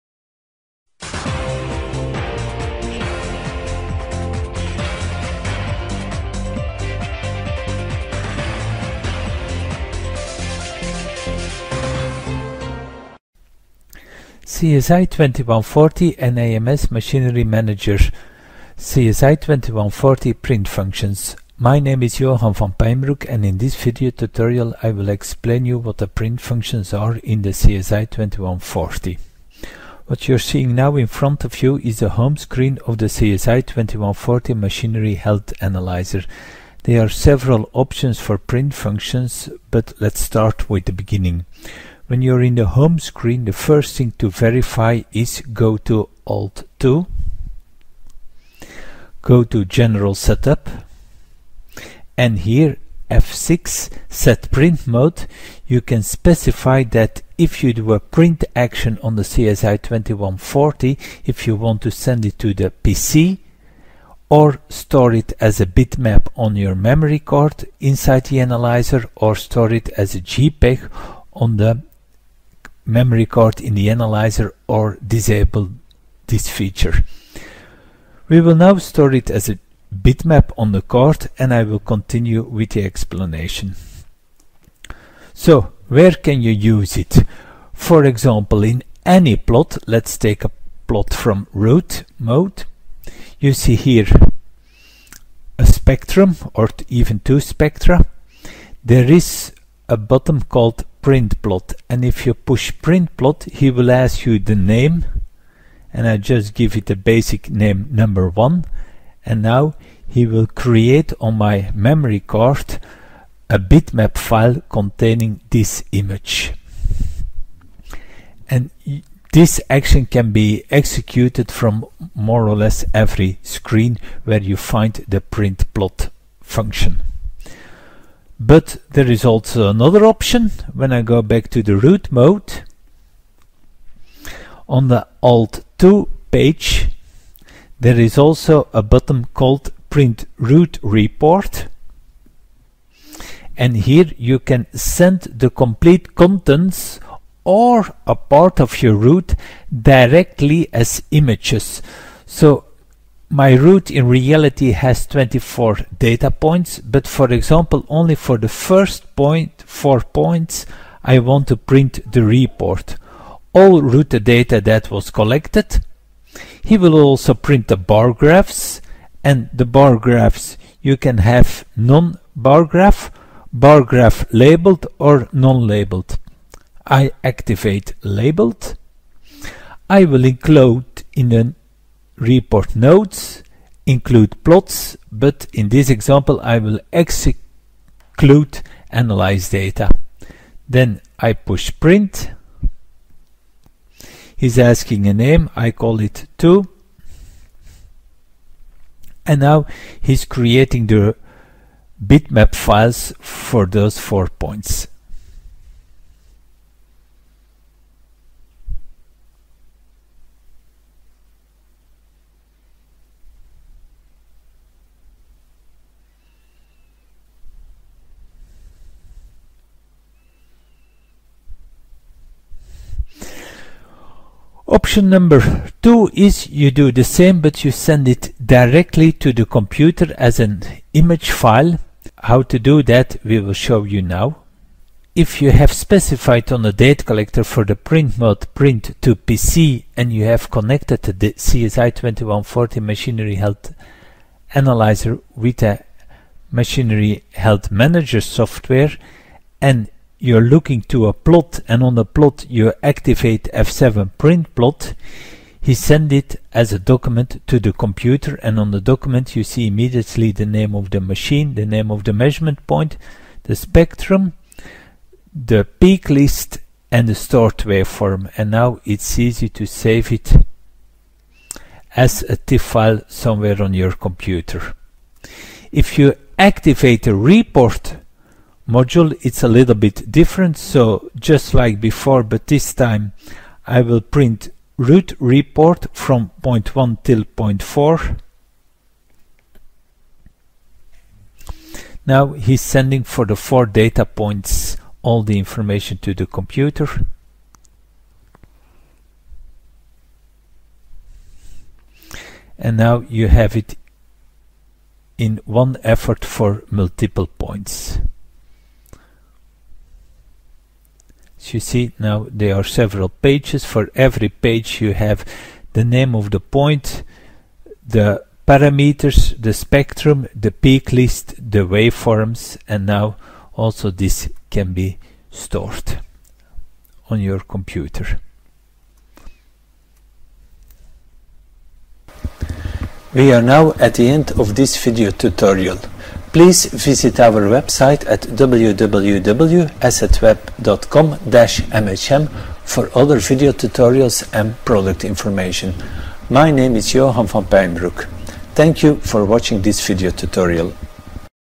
Print Functions of the AMS 2140 Vibration Analyzer
Demonstrates the print functions of the AMS 2140.